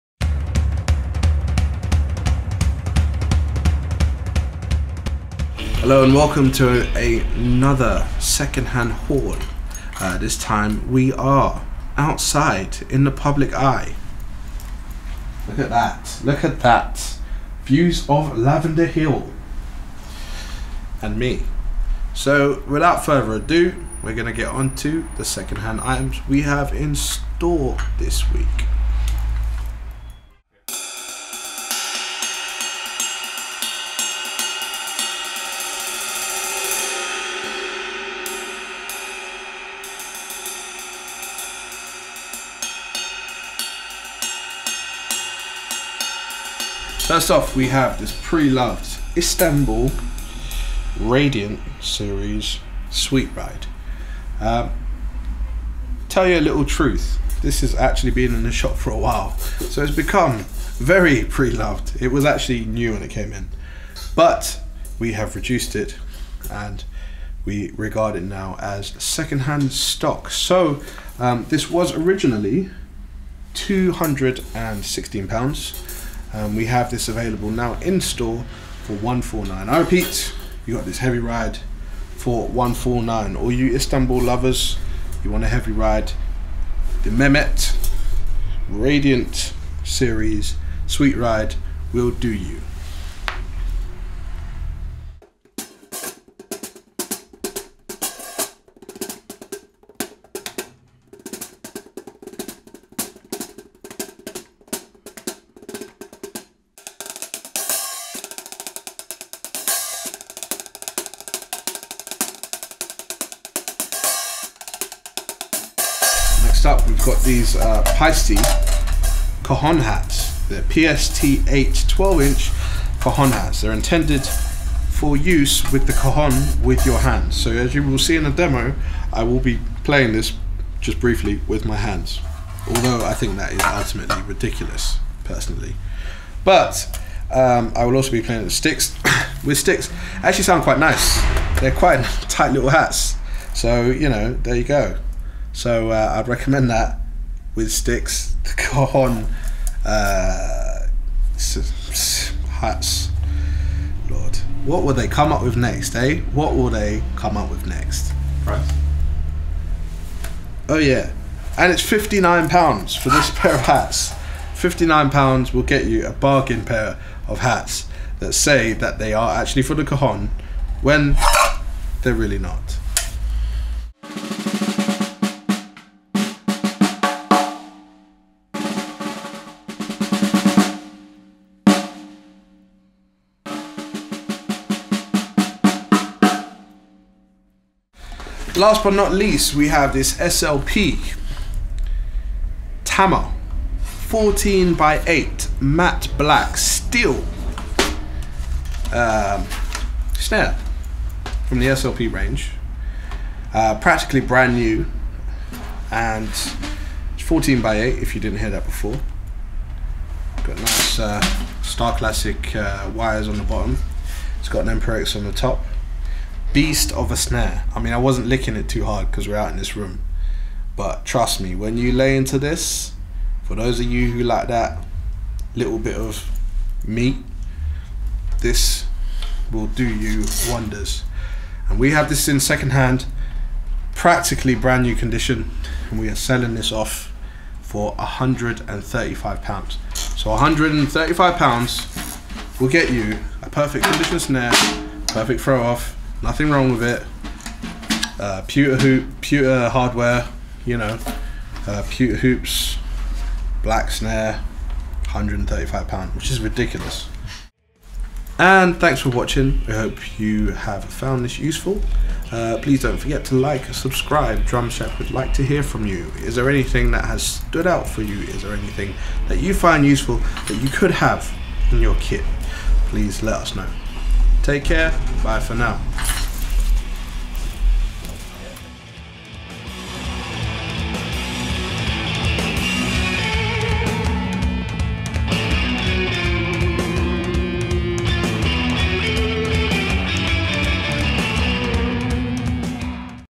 Istanbul Mehmet Radiant 22" Sweet ride, Paiste 12" Hi-hats and Tama SLP Snare 14" x 8" - Weekly #58
Istanbul Mehmet Radiant 22" Sweet ride, Paiste Cajon 12" Hi-hats and Tama SLP Snare 14" x 8" - Weekly #58

We are back with our weekly videos! Check out these amazing 2nd hand goodies that have just arrived in the shop!

Istanbul Mehmet Radiant Series Sweet Ride (22") - 0:31
Pre-loved at £149.00

Paiste PST8 Cajon Hi-Hats (12") - 1:37
Pre-loved at £59.00

Tama SLP Steel Snare (14" x 8") - 2:57
Pre-loved at £135.00